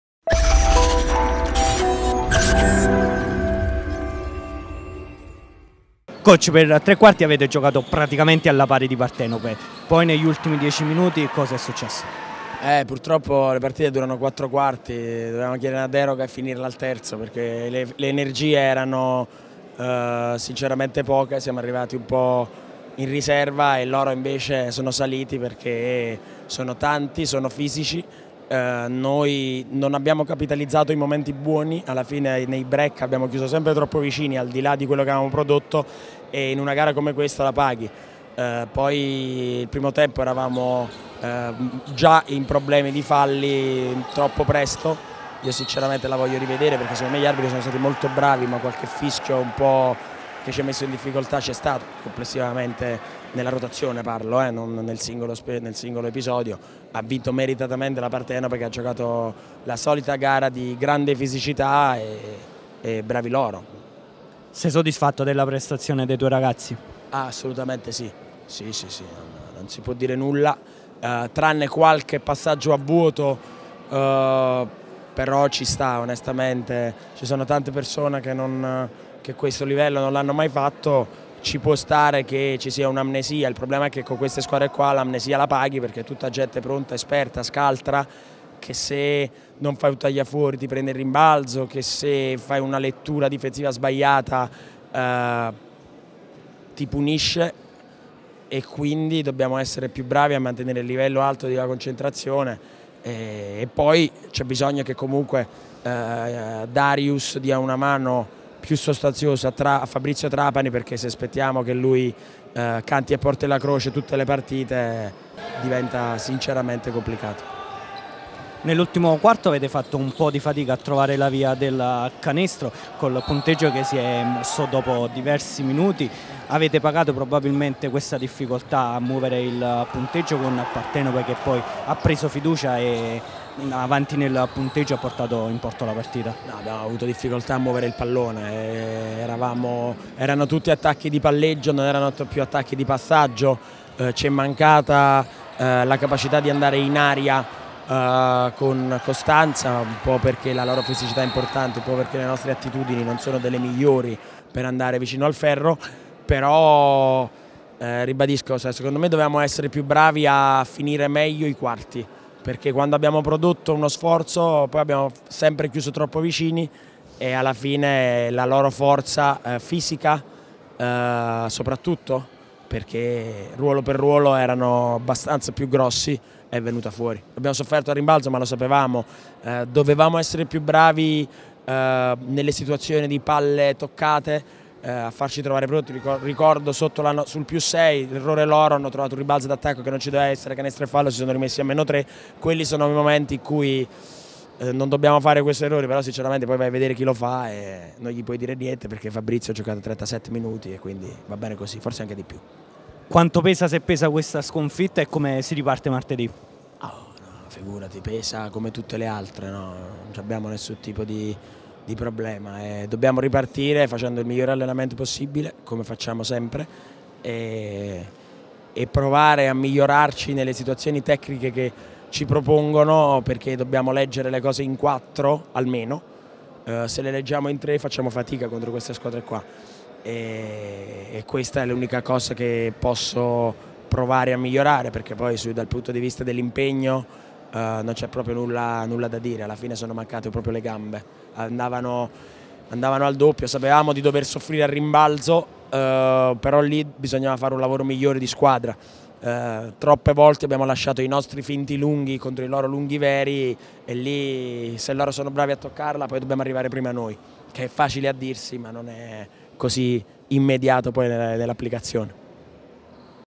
18° giornata: intervista post-partita a coach Sanfilippo
Il tecnico Nino Sanfilippo parla al termine della partita persa contro la Pallacanestro Partenope.